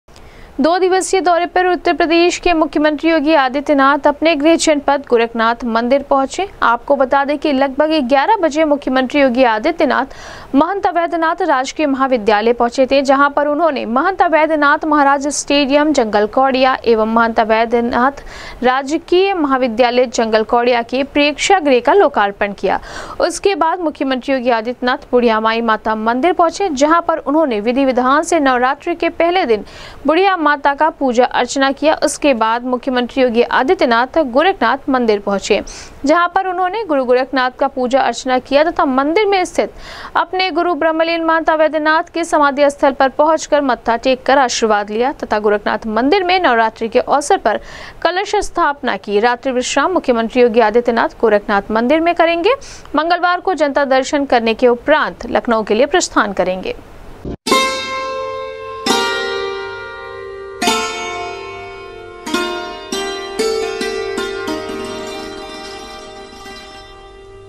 दो दिवसीय दौरे पर अपने गृह जनपद गोरखपुर पहुंचे उत्तर प्रदेश के मुख्यमंत्री योगी आदित्यनाथ, नवरात्रि
दो दिवसीय दौरे पर अपने गृह जनपद गोरखपुर पहुंचे उत्तर प्रदेश के मुख्यमंत्री योगी आदित्यनाथ, नवरात्रि के अवसर पर गोरखनाथ मंदिर में कलश स्थापना के साथ विभिन्न कार्यक्रम में हुए शामिल,गोरखनाथ मंदिर में करेंगे रात्रि विश्राम, मंगलवार को जनता दर्शन करने के उपरान्त लखनऊ के लिए करेंगे प्रस्थान